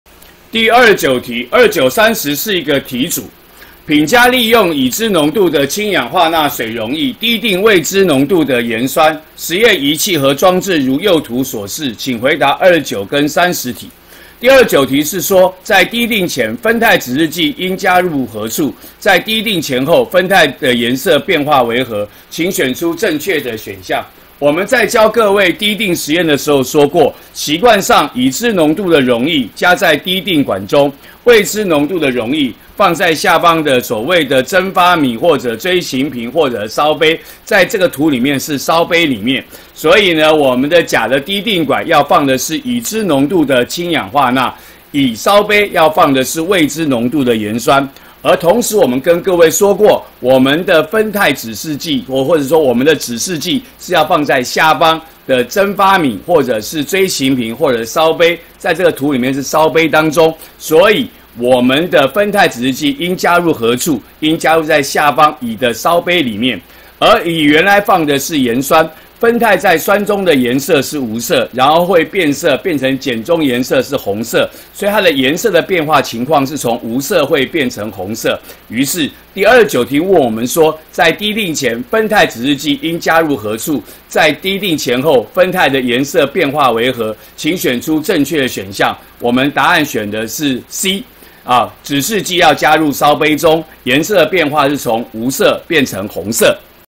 104學年下8年級理化二段第29題
問氫氧化鈉滴定鹽酸，指示劑放在何處，顏色如何變化。